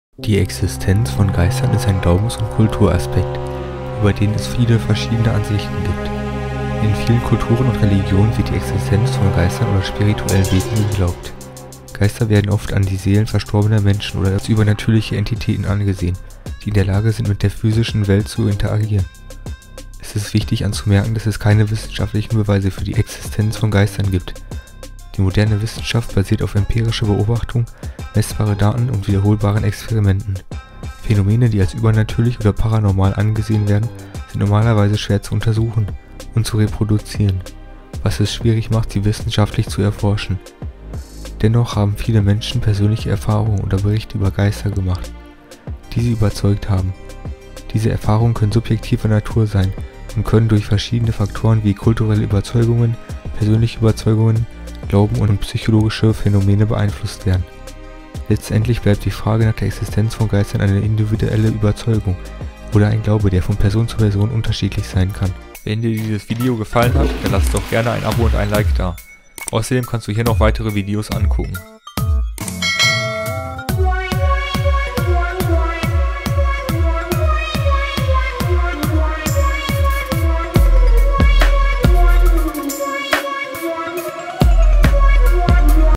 Gibt es Geister?
In diesem Video wird geklärt ob es Geister gibt.